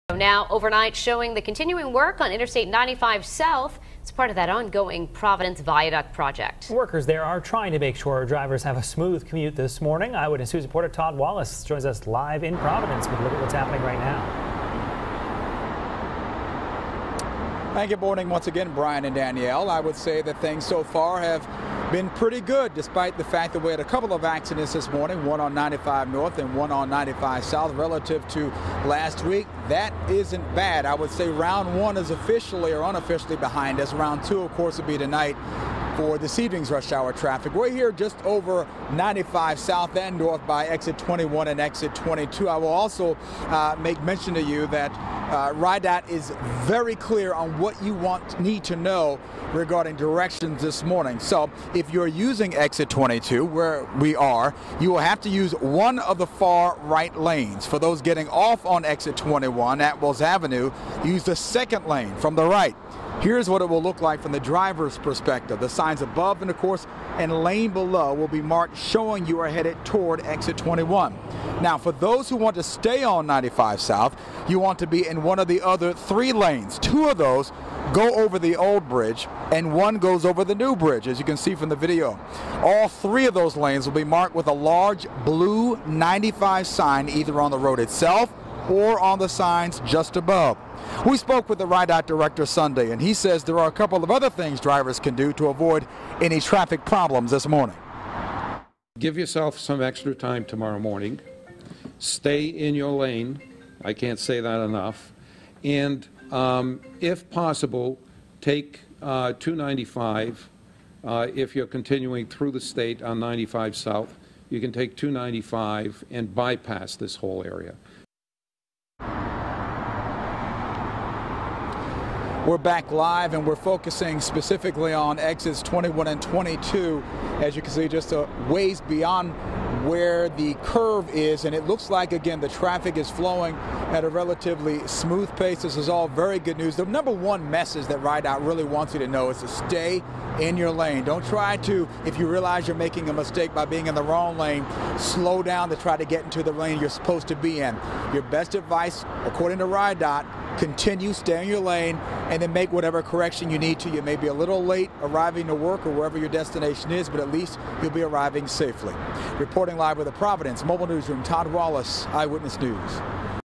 RIDOT: Stay in your lane through viaduct construction zone
RIDOT is re-introducing the lane changes as part of the construction work on the Providence Viaduct project.